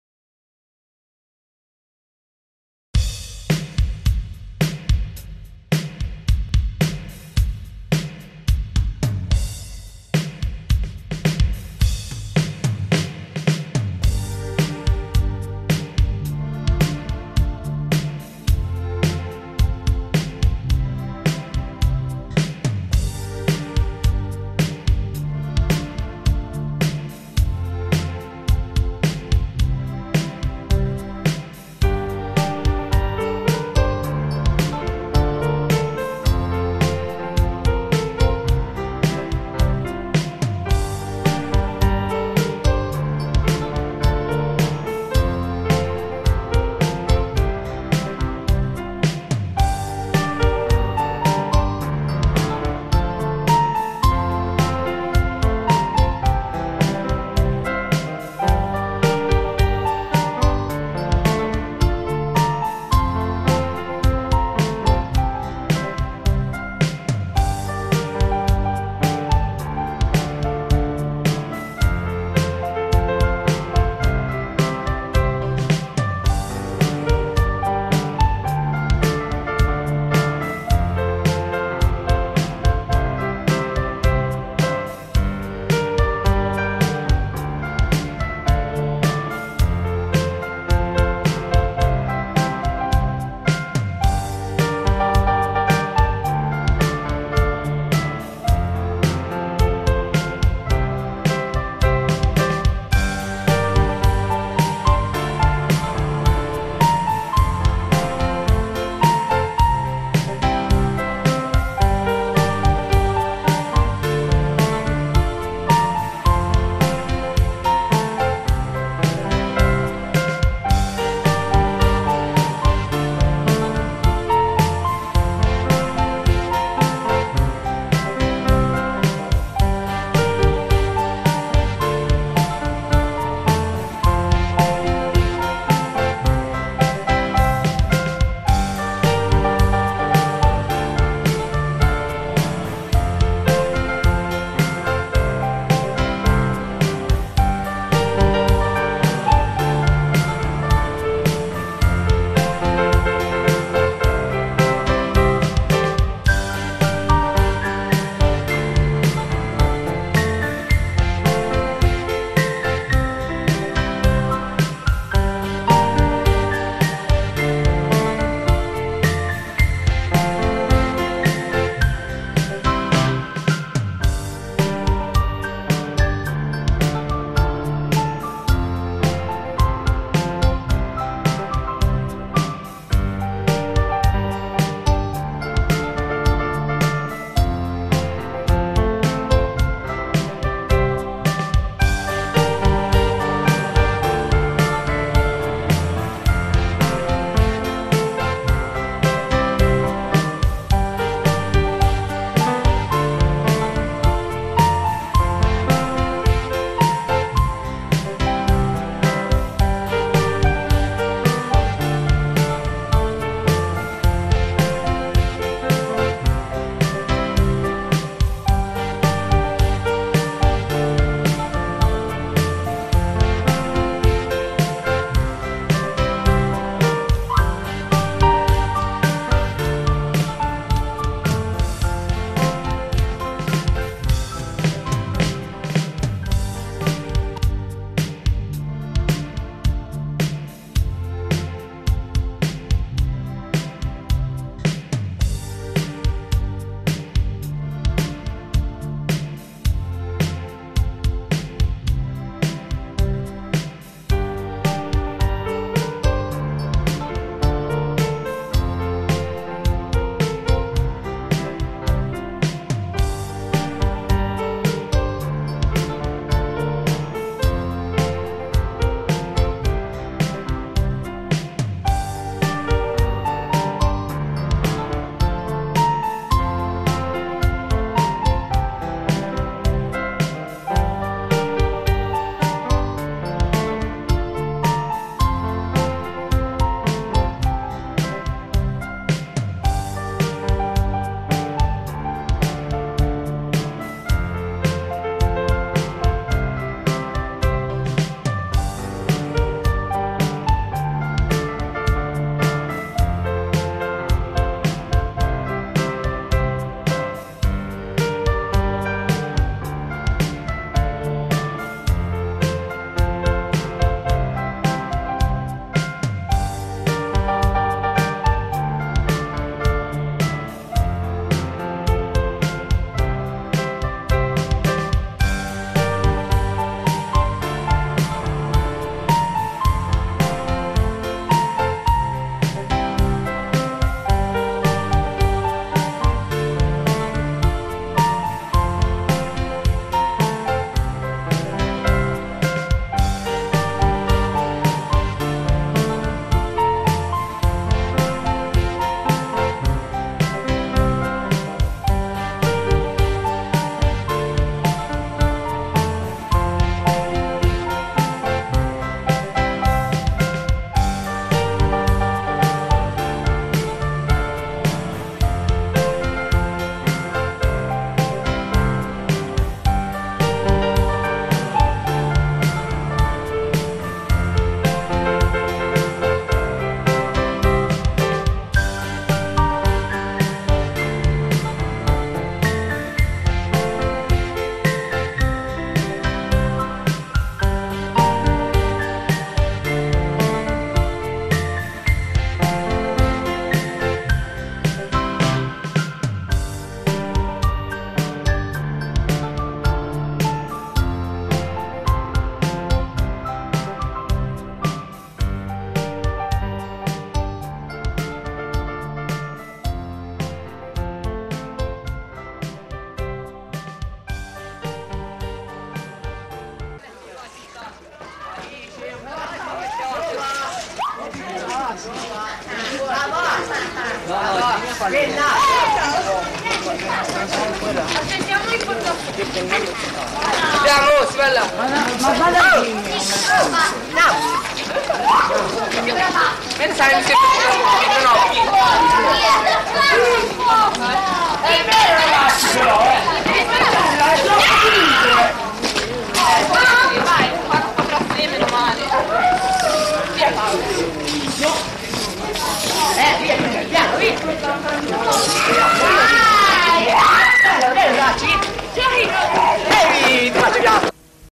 LA NEVE NEL 2001 - MONTECERBOLI
Bellissimo ricordo filmato da RUM della neve nel 2001 a Montecerboli!
VI RICORDATE QUANTA NEVE? FANTASTICO!